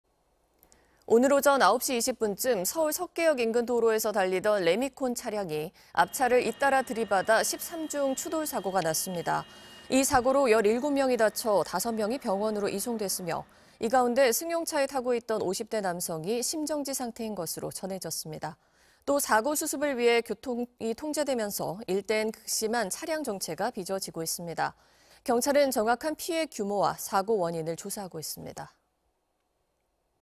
오늘 오전, 서울 석계역 인근 레미콘 차량 13중 추돌사고‥17명 부상 (2024.03.29/12MBC뉴스)
오늘 오전 9시 20분쯤 서울 석계역 인근 도로에서 달리던 레미콘 차량이 앞차를 잇따라 들이받아 13중 추돌사고가 났습니다.